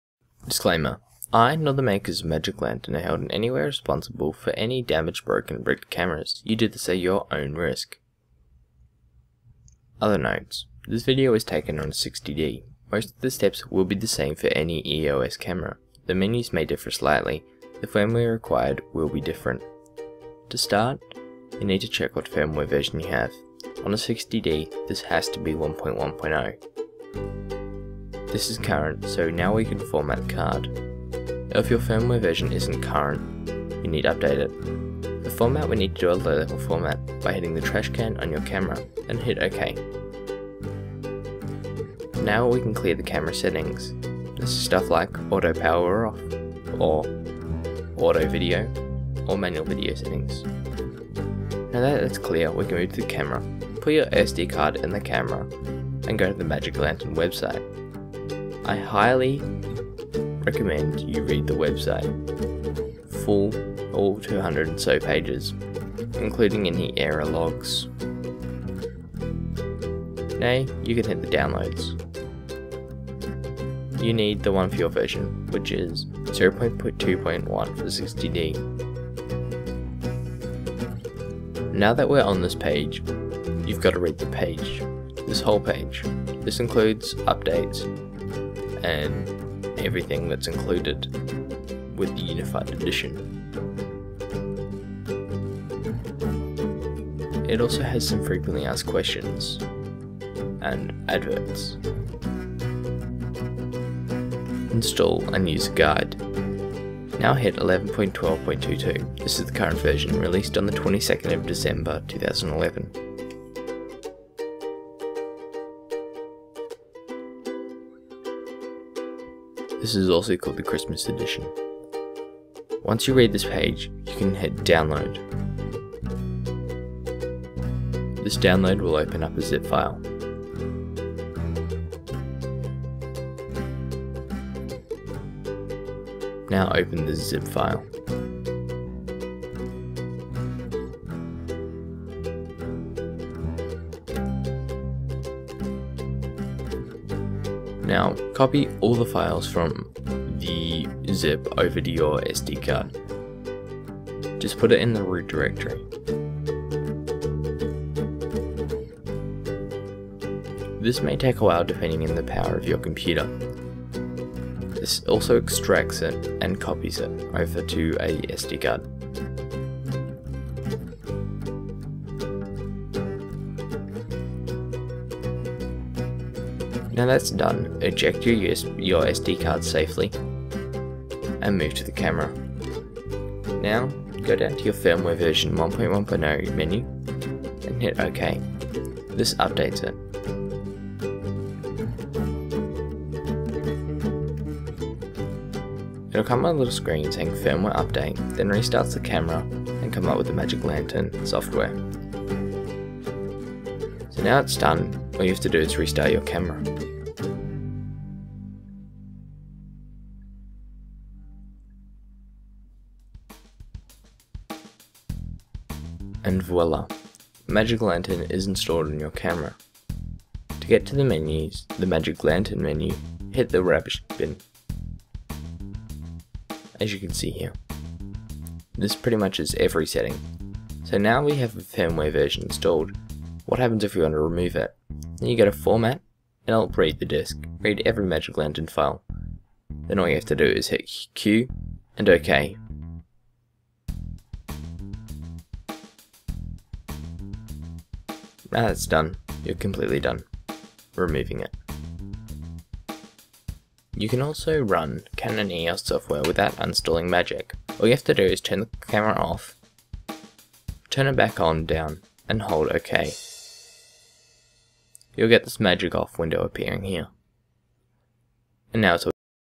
Install magic Lantern on EOS Camera / 60D
I never could find a decent video of how to install magic Lantern, So I Made one.

OTHER  NOTES
This  video  was  taken  on  a  60d. Most  of  the  steps  will  be  the  same  for  any  EOS  camera. The  menues may  differ slightly. The  firmware required WILL be different.

Music is my own, if you want it, comment and I'll post a link...Enjoy